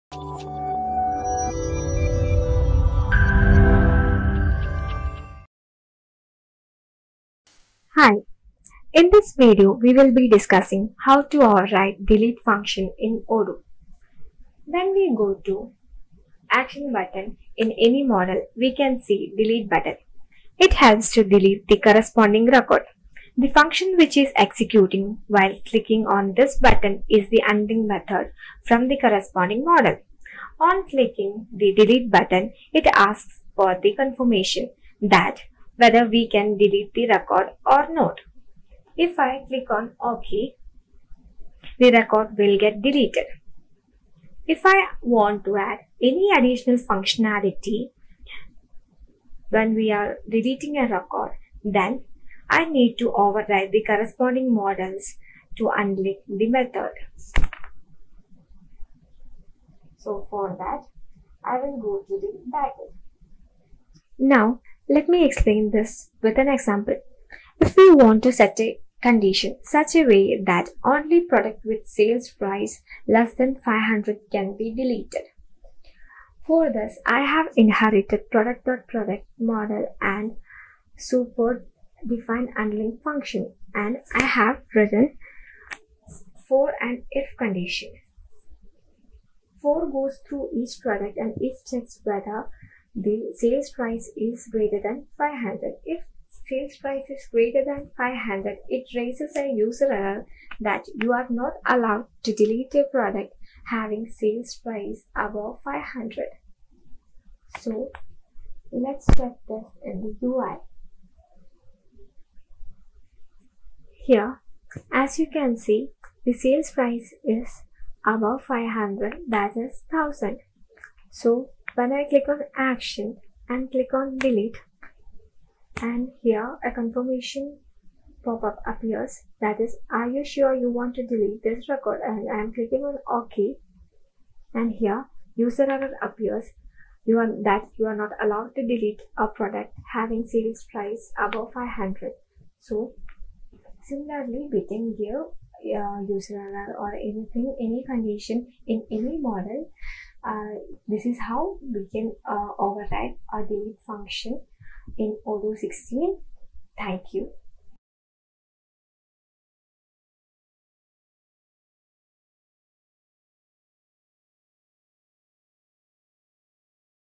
How to Override Delete Function in Odoo 16 | Odoo Development Tutorials
This video explains how to override the delete function in odoo16. The function which gets executed on deleting a record is the unlink method of the model.

By overriding the unlink method of the corresponding model we can even add additional functionality while we delete a record.